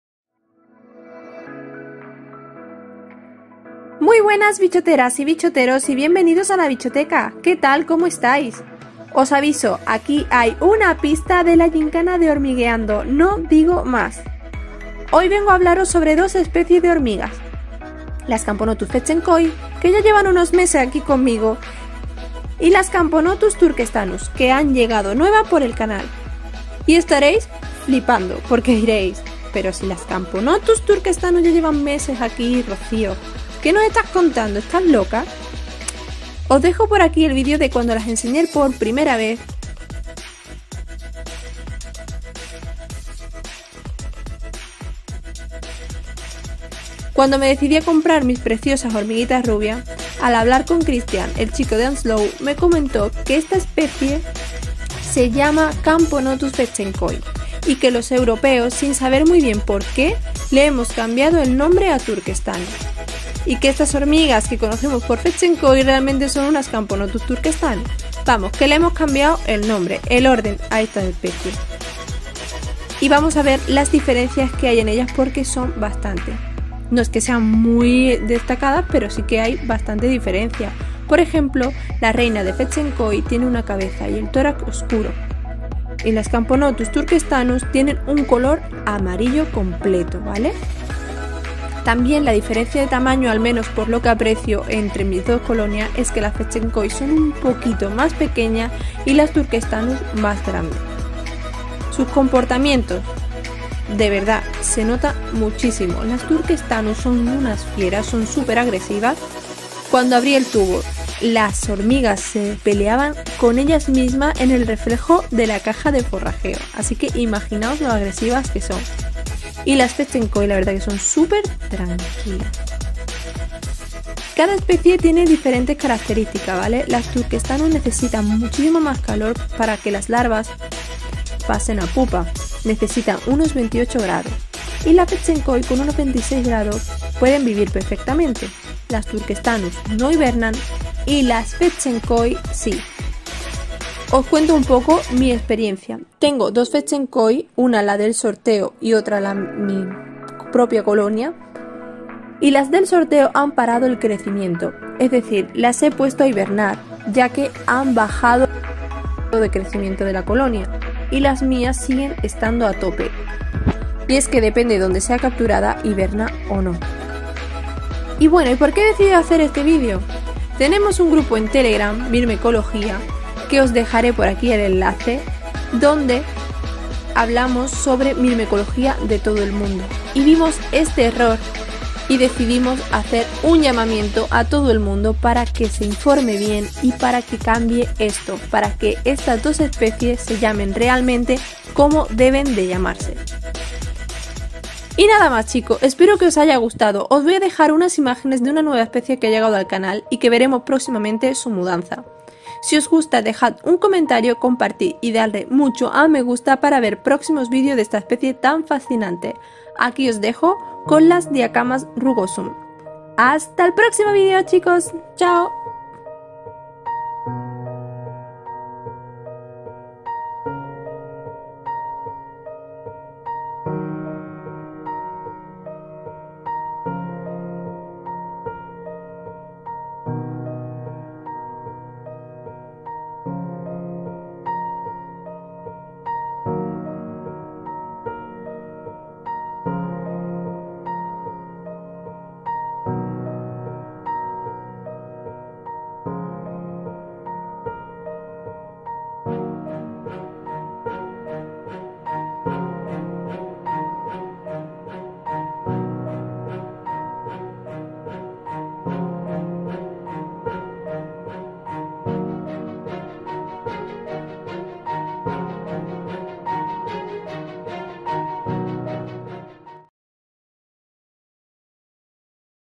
🕵️¿Quién es quién? Camponotus turkestanus y Camponotus fedtschenkoi
🧡 Las CAMPONOTUS más bonitas de ASIA. si queréis aprender a CRIAR estás hormigas tenéis que ver este vídeo🧡

os dejo por aquí el Instagram de Hormigueando para que vayáis a echarle un ojo y disfrutar de su gyncana